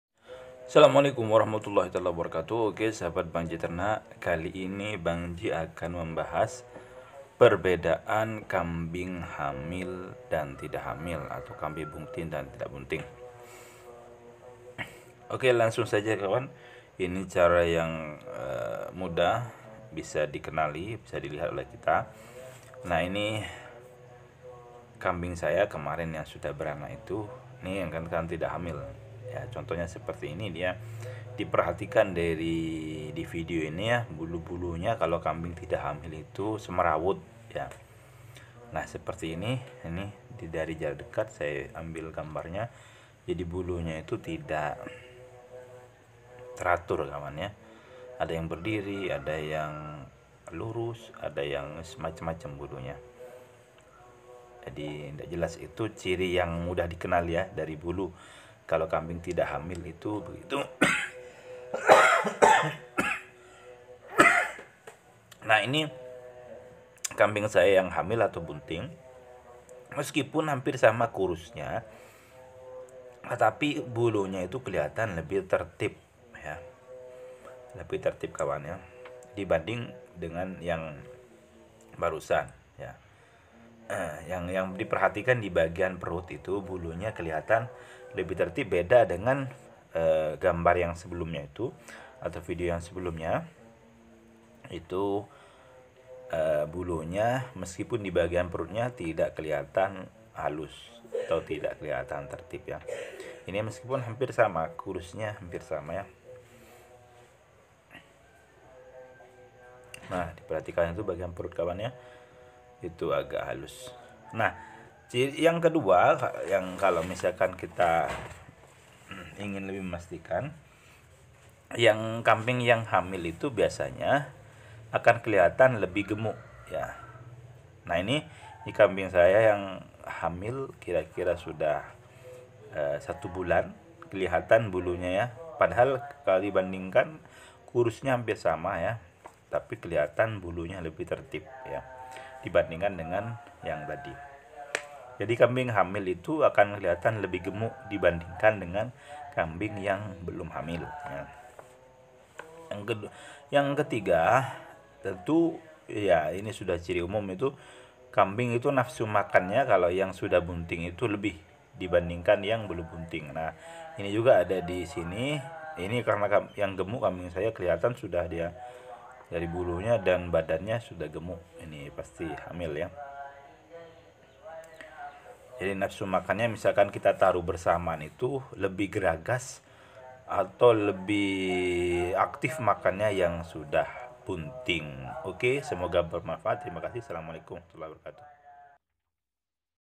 perbedaan kambing hamil dan tidak hamil
video ini membahas perbedaan kambing hamil dan tidak hamil